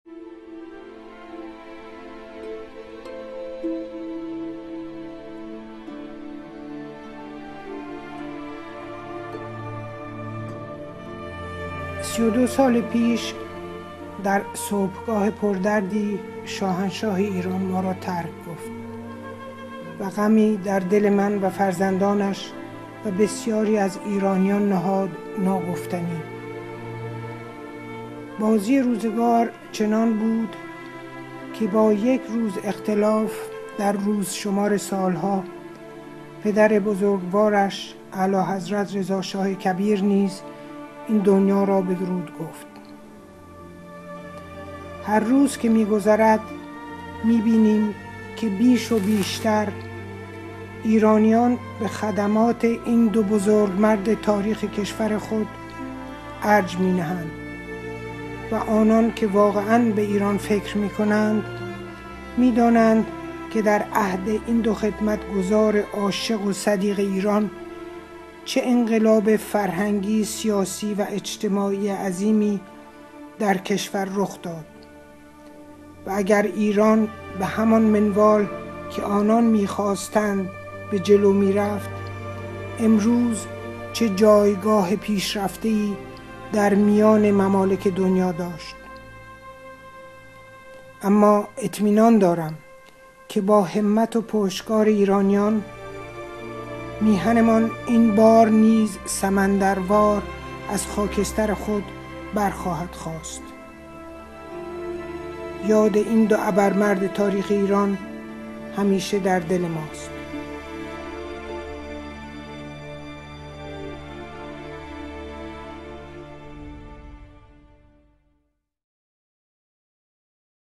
H.I.M. Farah Pahlavi in Remembrance of the Shahanshah of Iran
پیام شهبانو فرح پهلوی به مناسبت سی و دومین سالگرد درگذشت شاهنشاه محمدرضا پهلوی